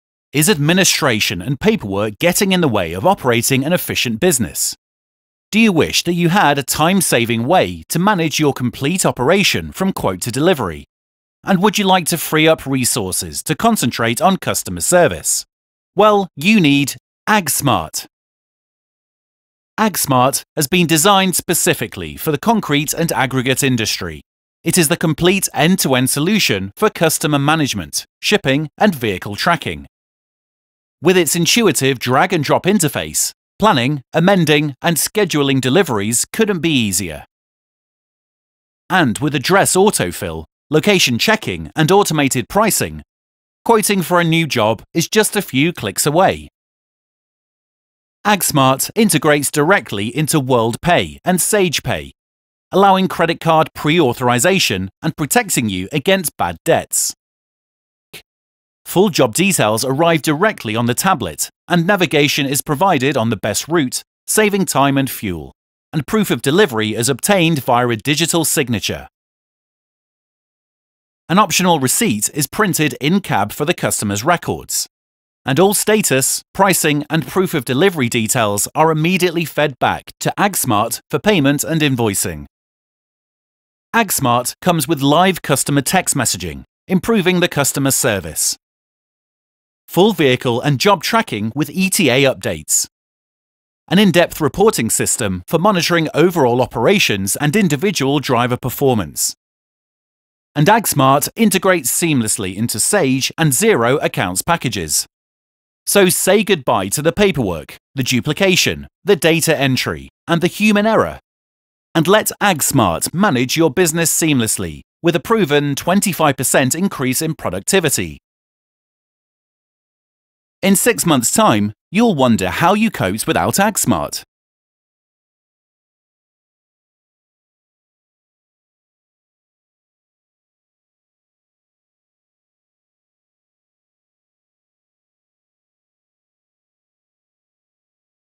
AGG SMART - Ready Mix Concrete Shipping System - 0116 298 2888
AGG SMART is an integrated "cloud based" shipping system developed specifically for the concrete and aggregate sectors. It is a complete "end to end" solution, managing everything from customers, to quotes, to job planning, to delivery tracking, to invoicing and reporting.

"AGG SMART has revolutionised our office and allowed us to reduce office labour hours" - Bobby Saundh - Rock Solid Concrete